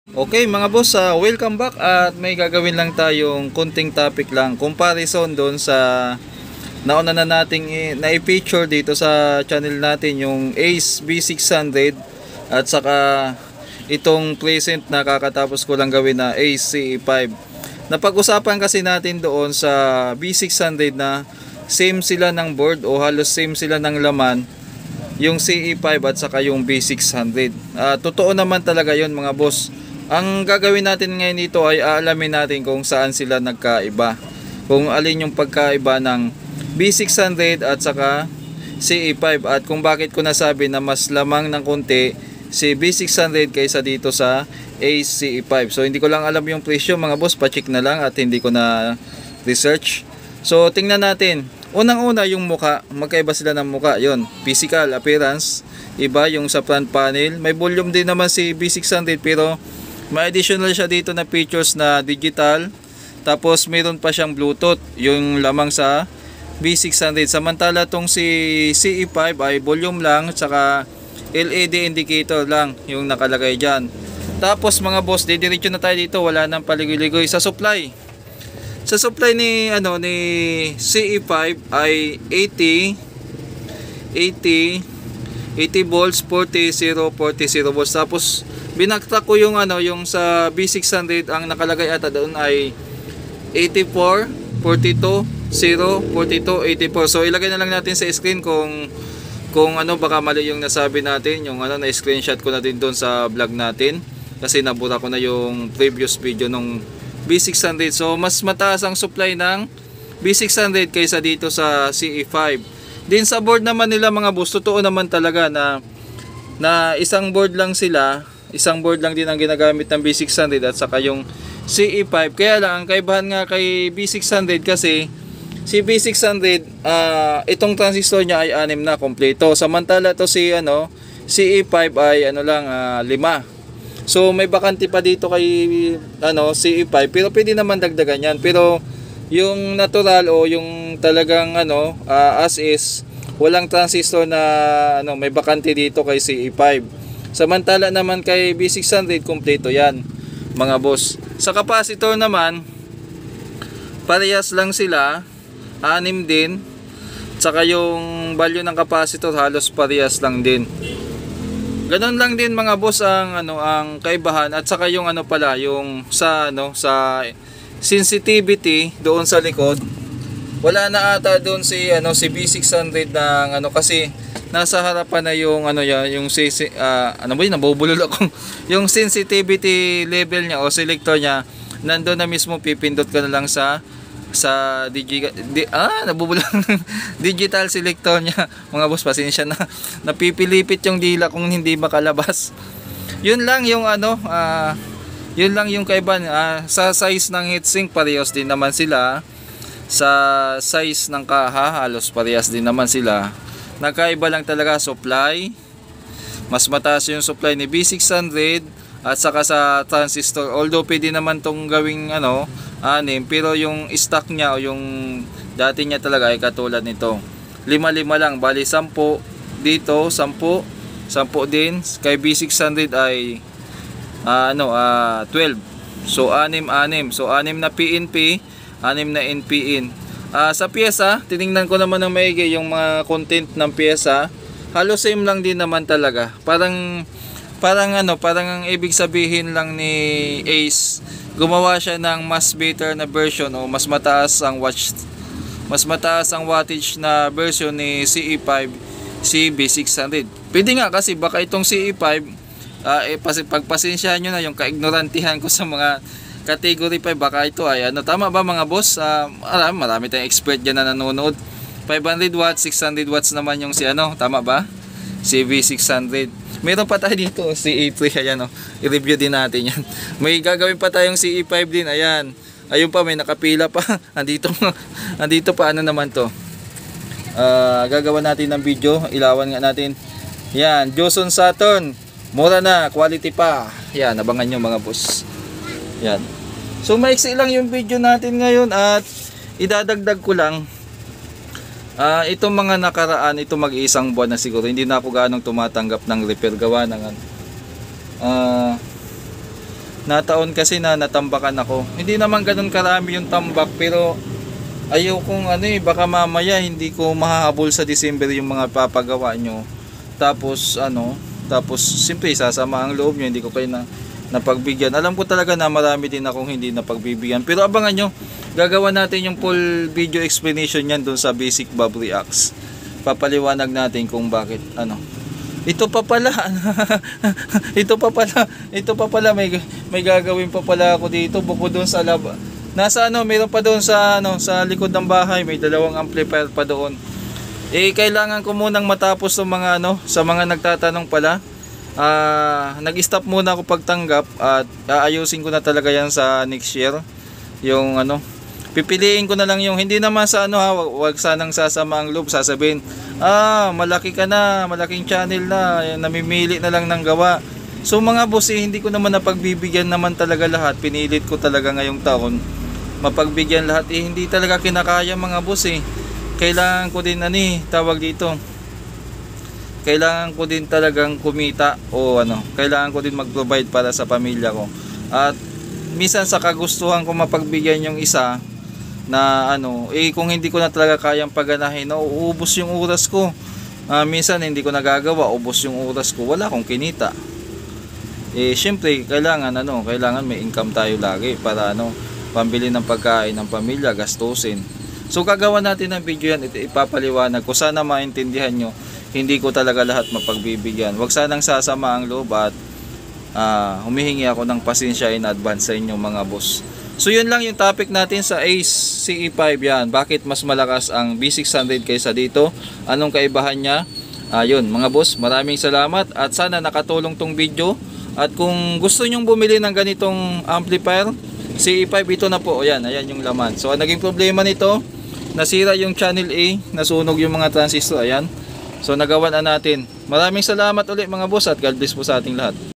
Bakit mas lamang si Ace V600 kay Ace CA5 power amplifier
mga boss review natin sa ca5 at kumpara narin natin sa ace v600 power amp